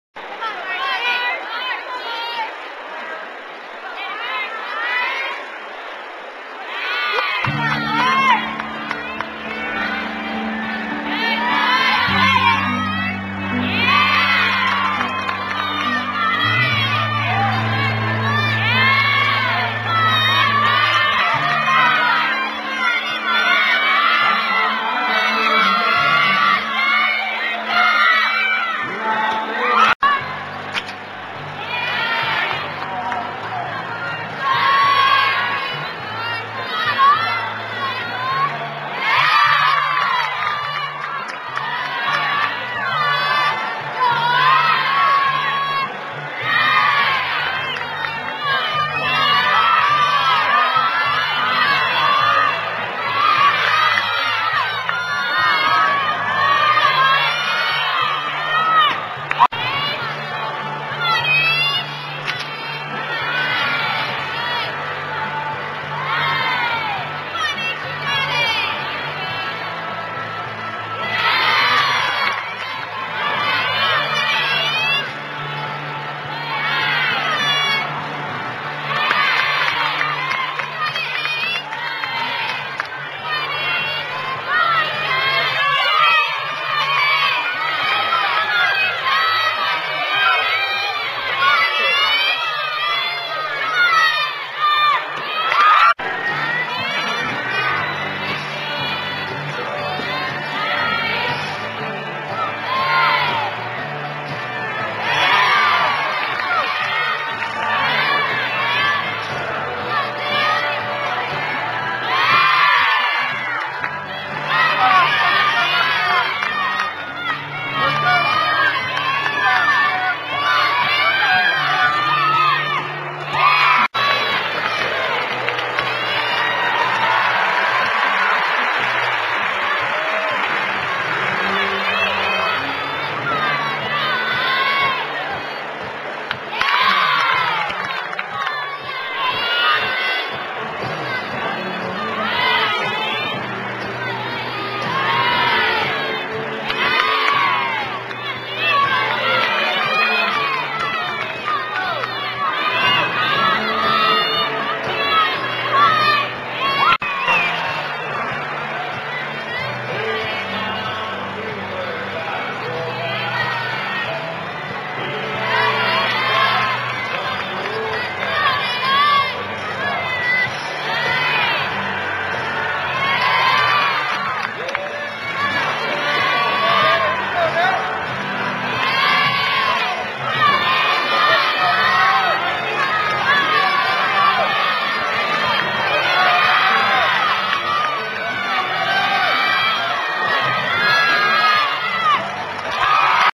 UCLA Gymnastics at Utah - Uneven Bars Highlights
Highlights on uneven bars from UCLA's dual meet at Utah (1/9/09)